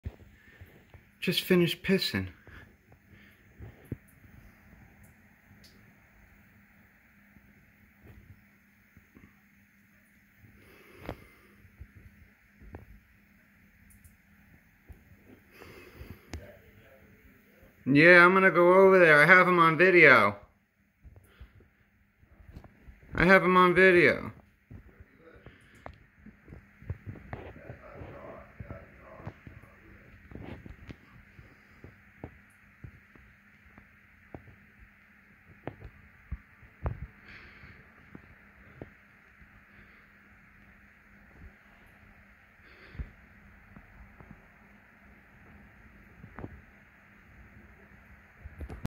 Guy Pissing In public Diring Coronavirus Part 1
Guy Pissing In public Diring Corona I'm Pretty Bored aAs You Can See Sorry I Cut The First Couple Second Due To Cursing